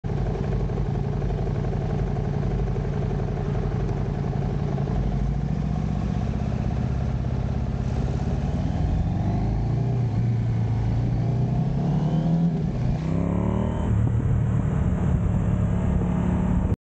Red Light Jumper
Not Close, but would he jumped it later and not seen the cyclist and myself?